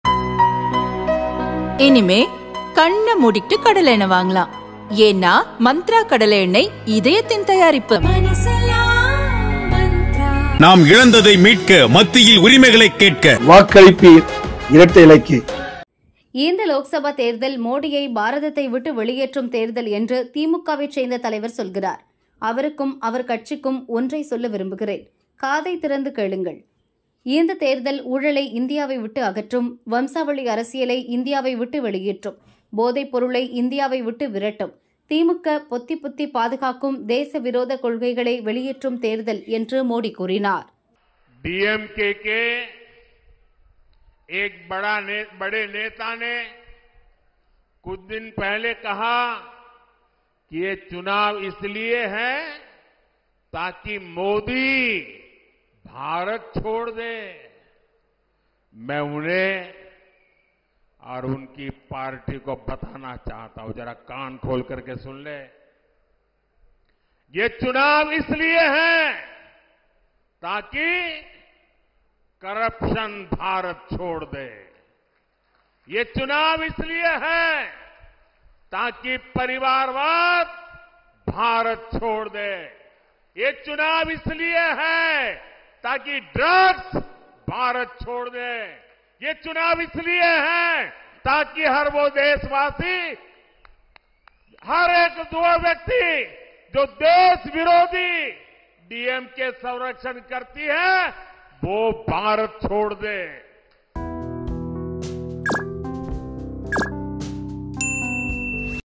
கோவை கூட்டத்தில் திமுகவுக்கு மோடி பதில் PM Modi| MK Stalin| DMK| Dynasty
மோடியை வெளியேற்றும் தேர்தலா?
வாரிசு அரசியல்தான் ஓடப்போகிறது!

கோவை கூட்டத்தில்
திமுகவுக்கு மோடி பதில்
PM Modi| MK Stalin| DMK| Dynasty|